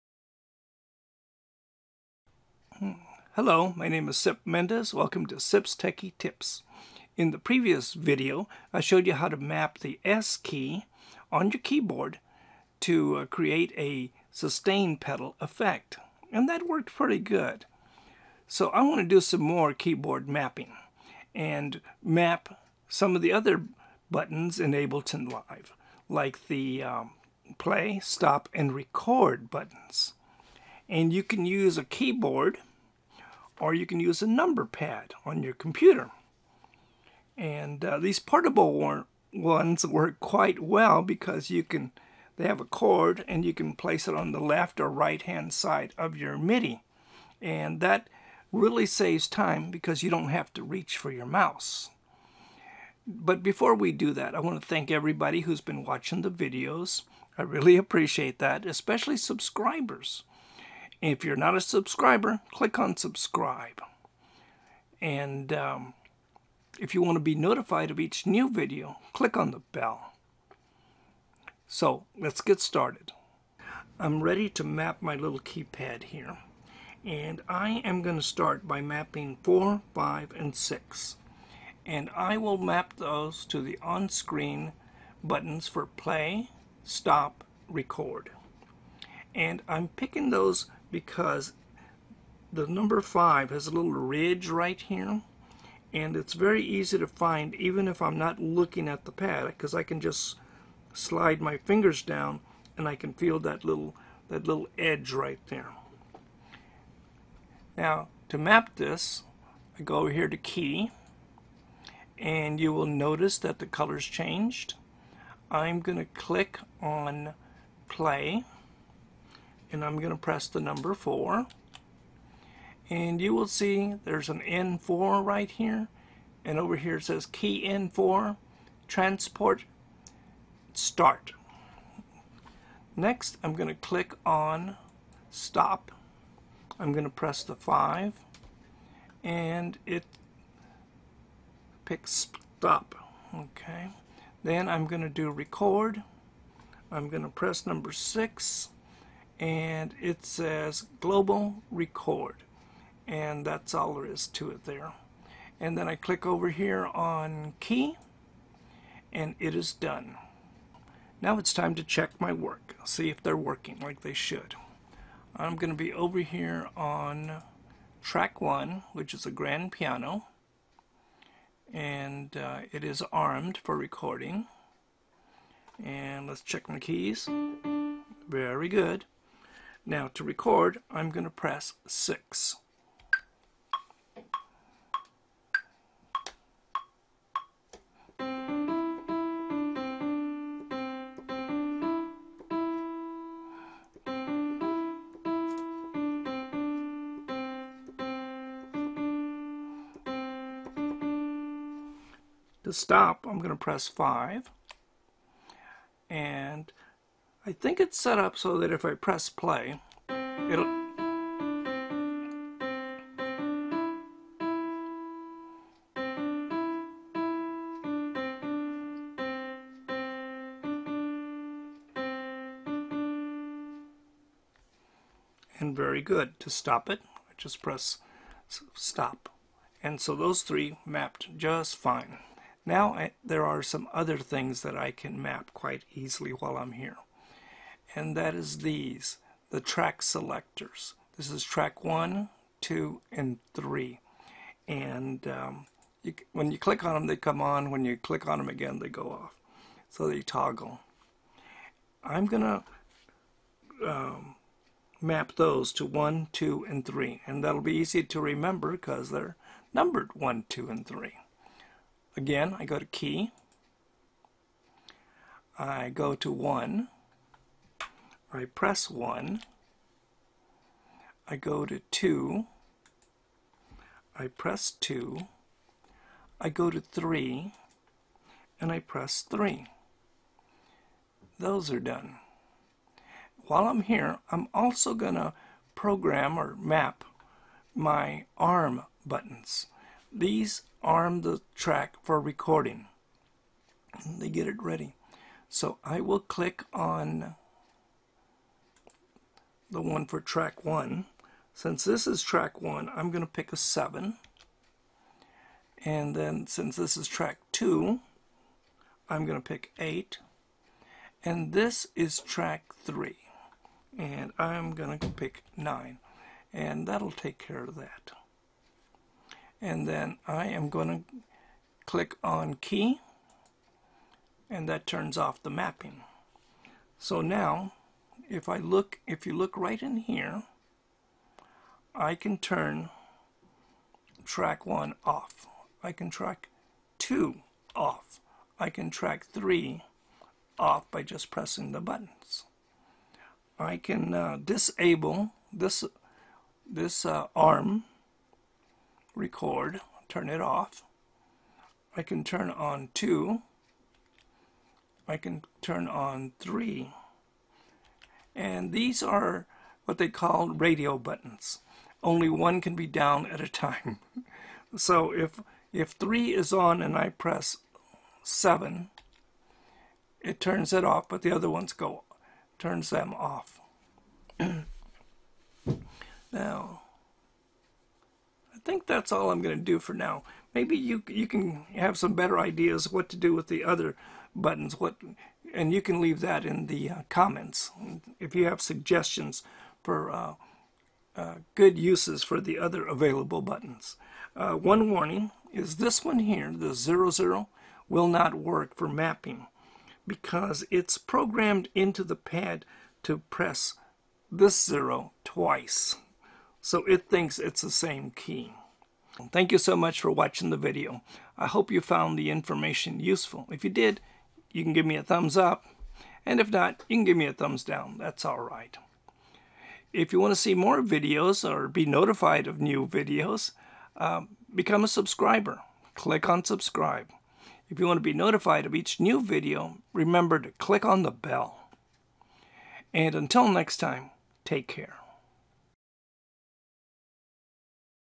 Transport Mapping in Ableton Live
This video will show you how to map numeric pad keys to Ableton Live 10 Transports. I will also show you how to program the Track Selector and Record Arm buttons.